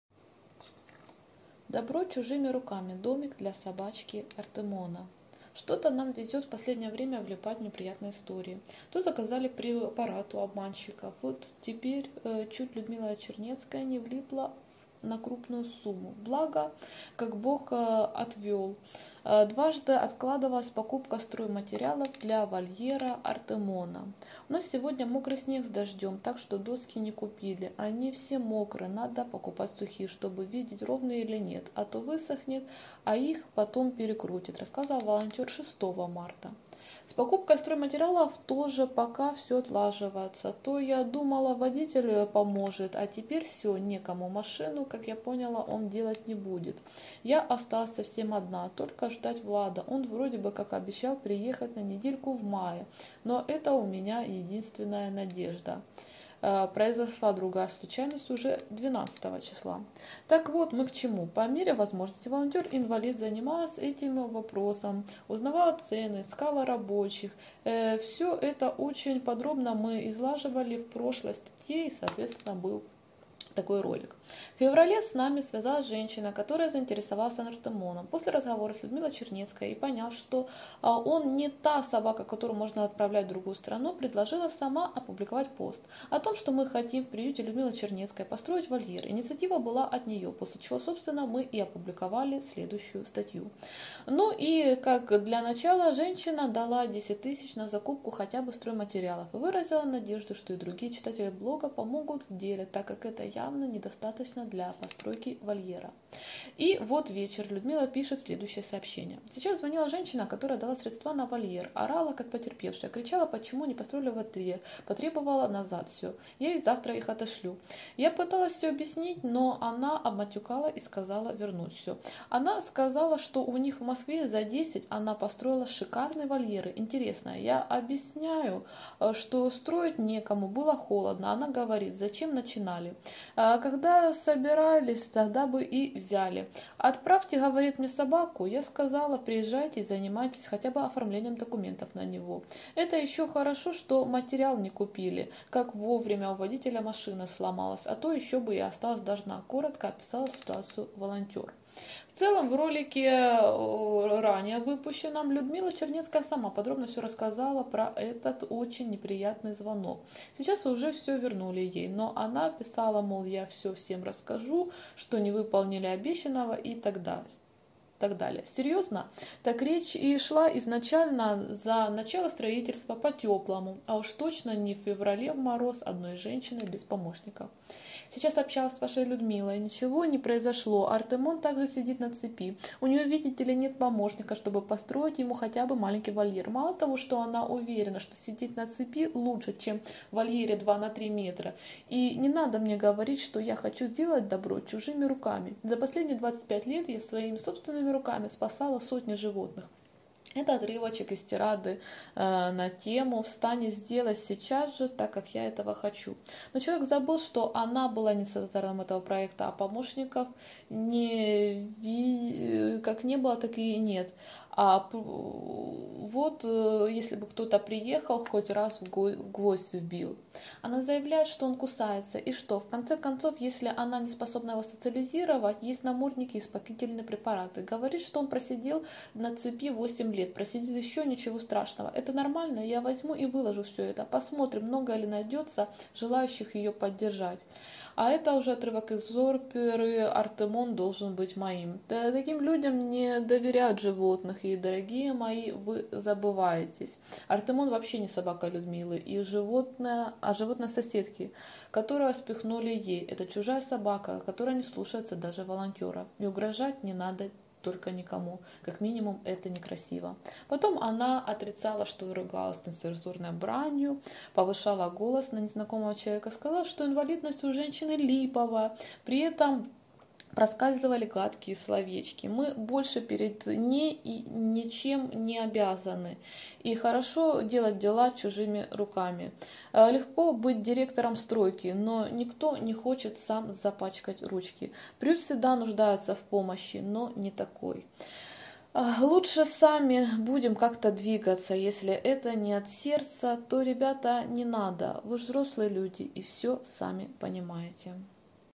Мечты собачки по кличке Артемон: как легко делать добрые дела...
Материал для ролика взят из статьи: "Добро чужими руками: домик для собачки Артемона"